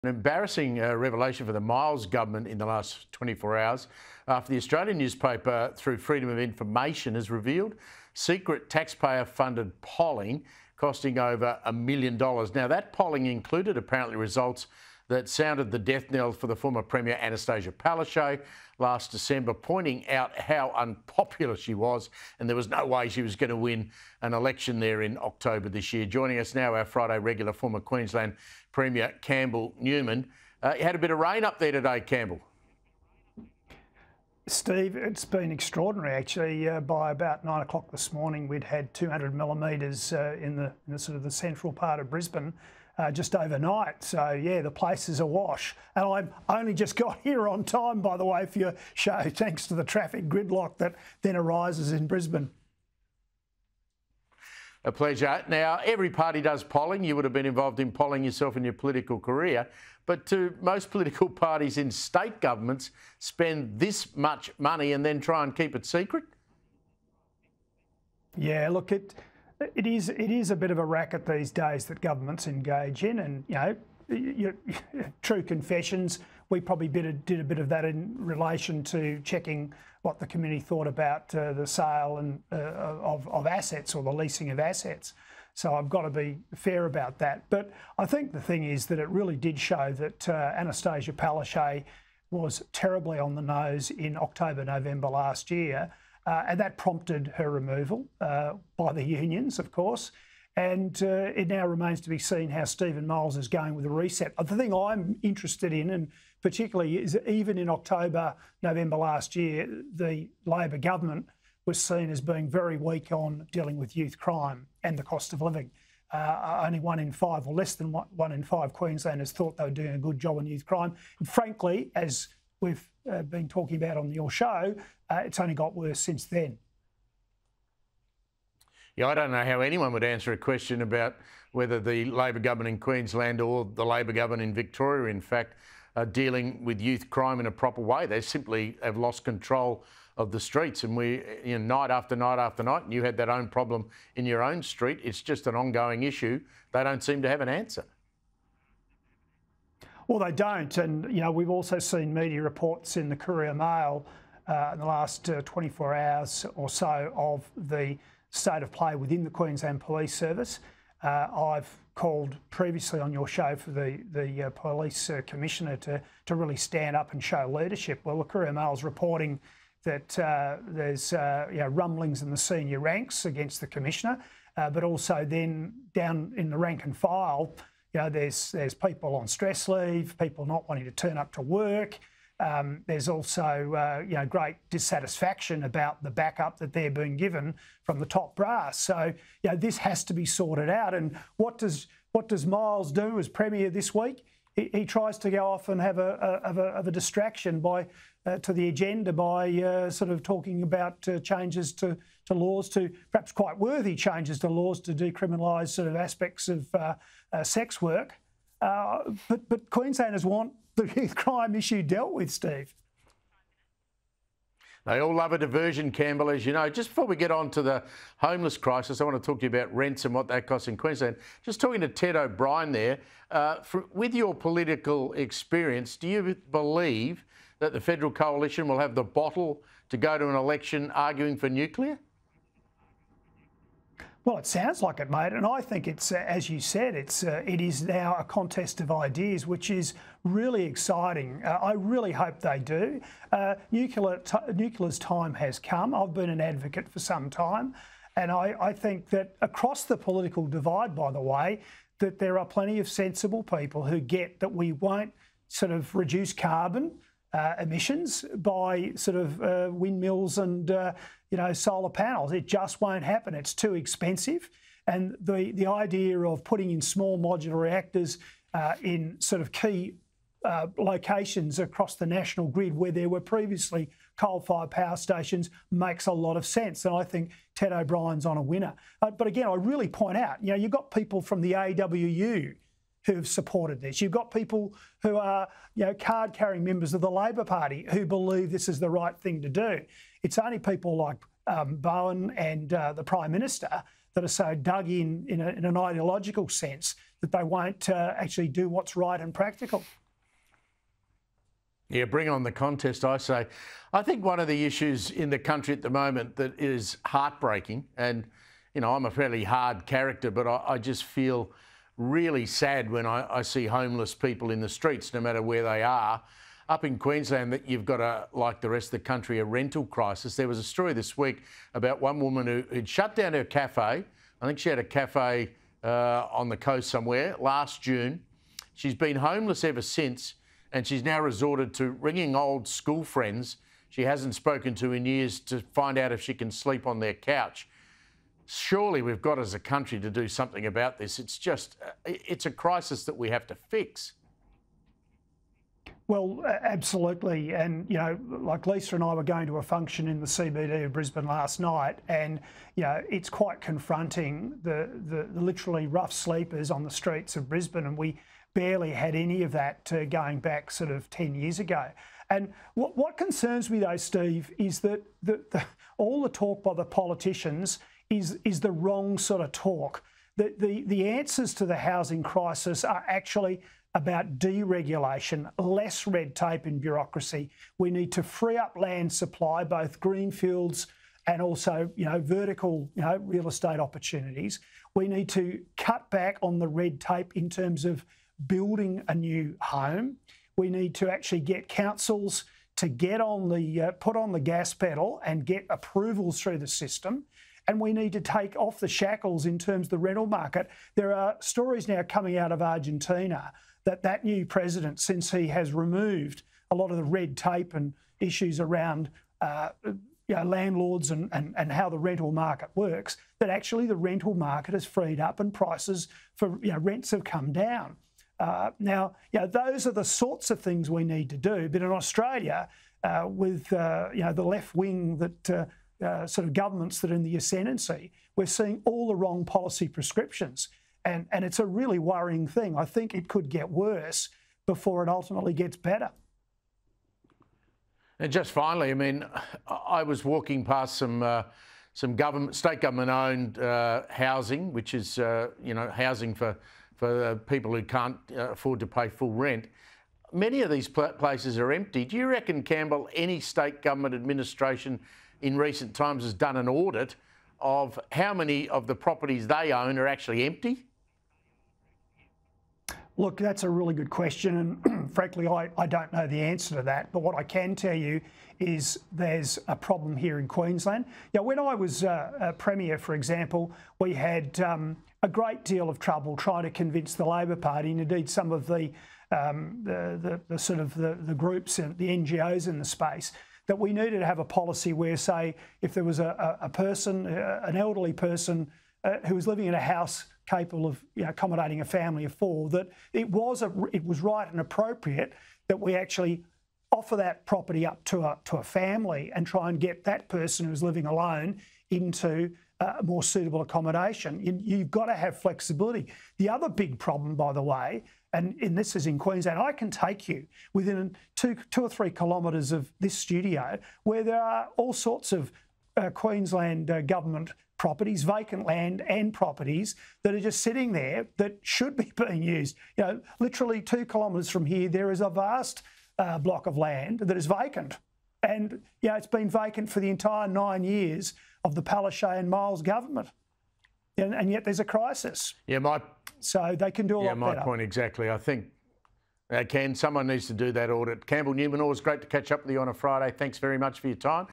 'Stand up and show leadership': Qld Police Commissioner faces rank and file 'rumblings'
Former Queensland premier Campbell Newman says he has called for Police Commissioner Katarina Carroll to “really stand up and show leadership” as the state faces a continuous youth crime wave.

Mr Newman pointed to a report stating “rumblings in the senior ranks” against the Commissioner as her workforce has begun to express “great dissatisfaction”.

“Down in the rank and file, there’s people on stress leave, people not being able to turn up to work,” he told Sky News Australia host Steve Price.

“There is also great dissatisfaction about the backup they are being given from the top brass.

“This has to be sorted out … Queenslanders want the real crime issue dealt with.”